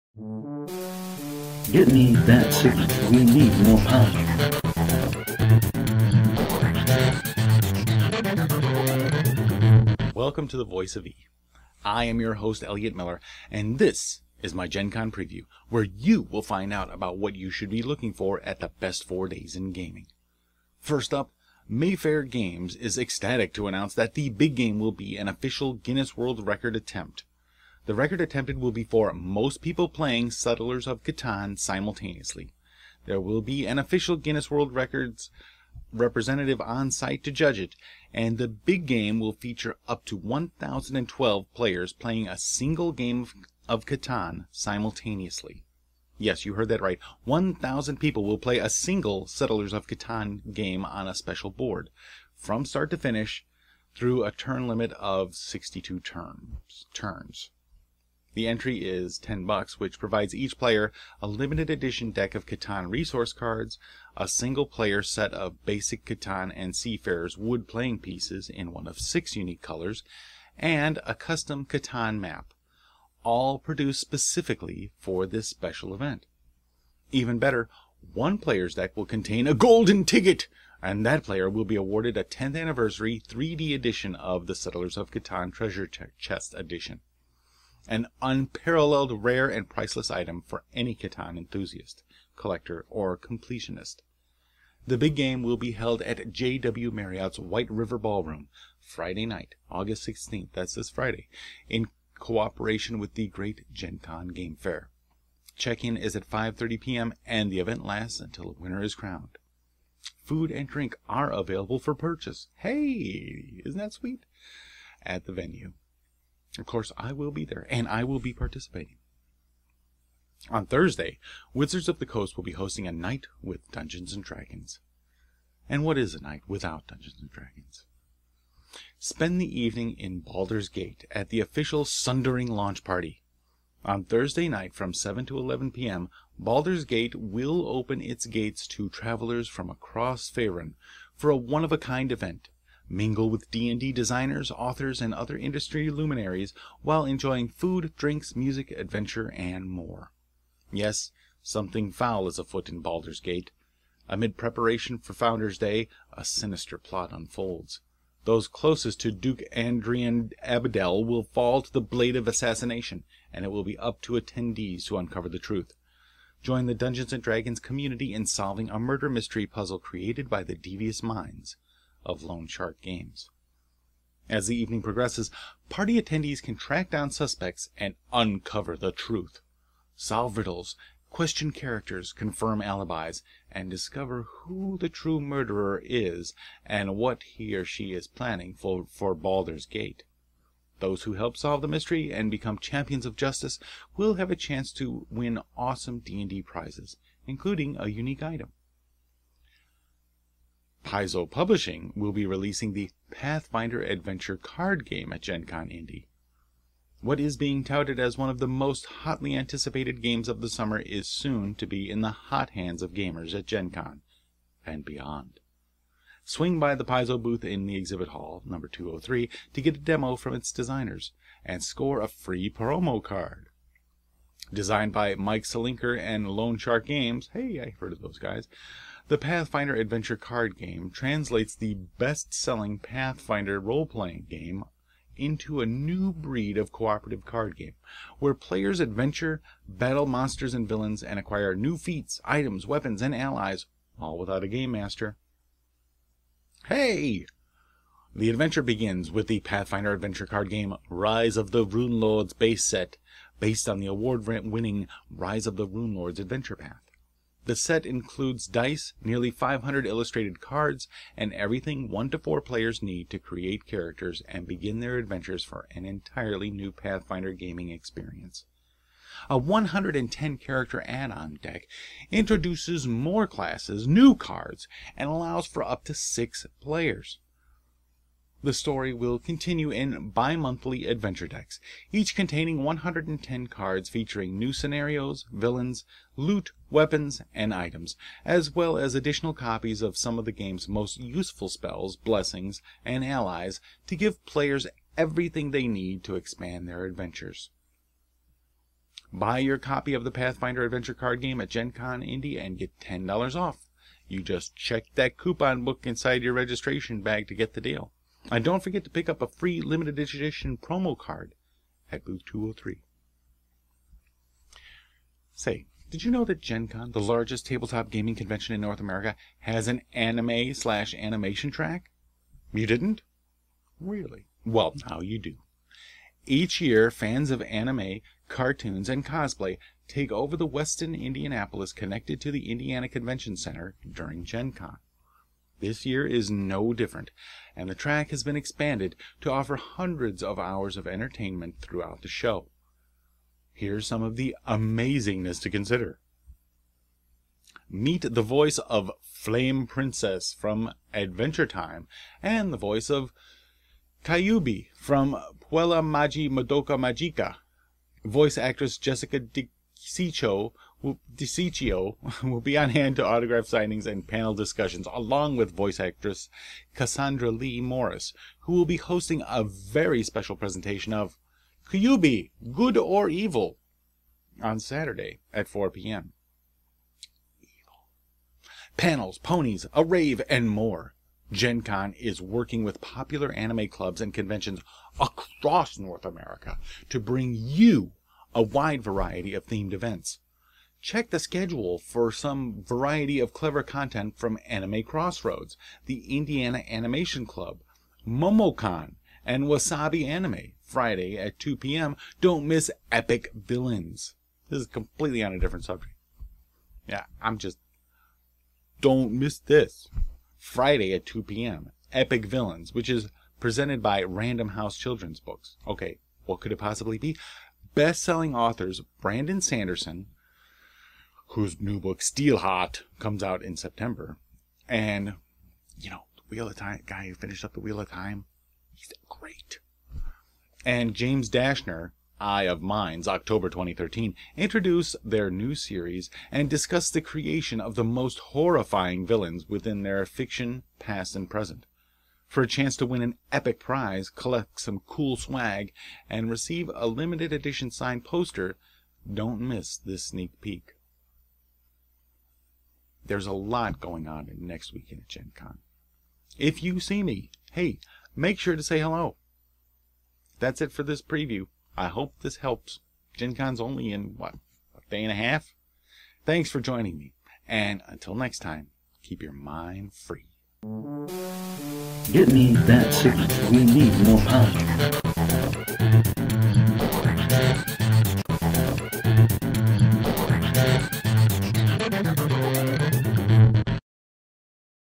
It's a Must See at Gen Con 2013
Elliott talks about some of the unique experiences available at Gen Con 2013. There will be so much to do that this is only a sample, but a good sample of events you might not know about. Wizards of the Coast, Paizo Publishing, Settlers of Catan, Anime and more.